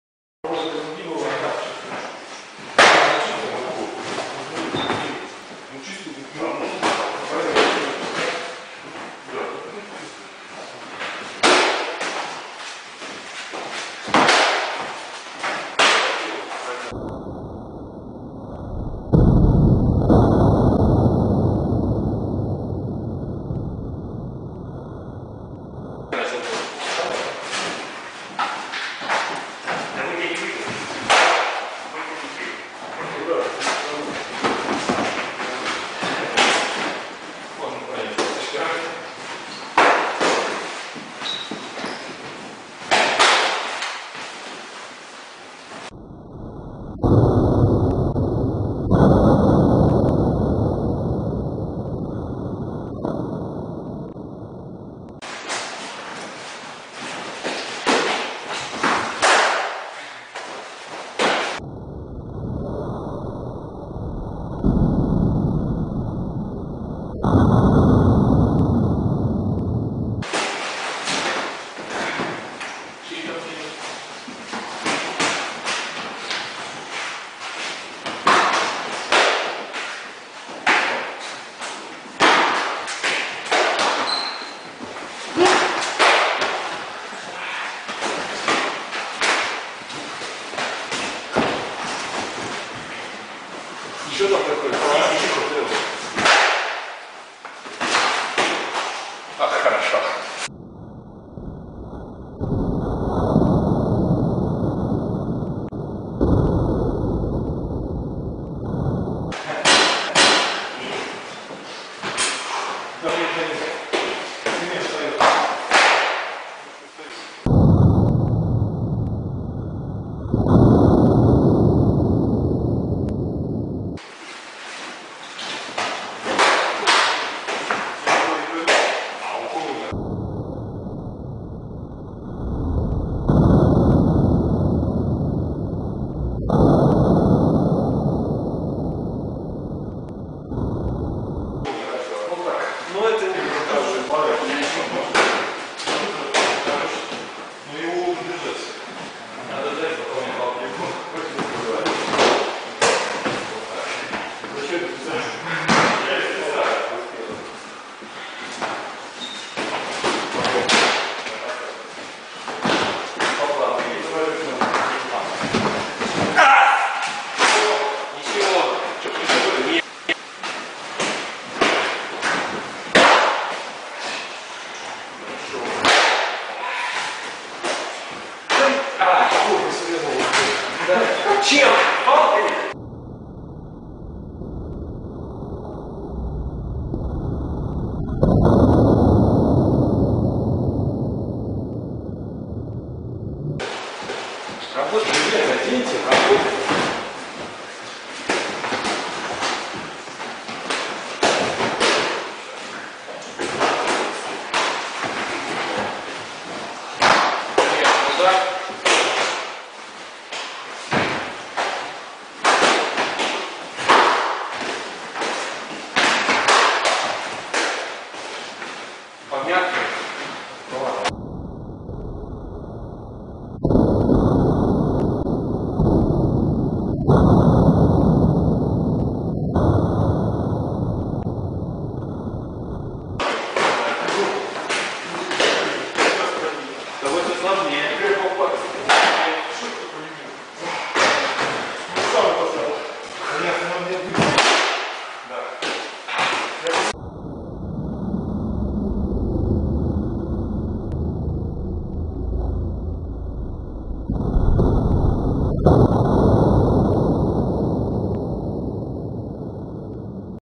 Stick fight. The Friends - Duellists (1)
Training in Arms Fight Club.  Fight and slow video replay episodes.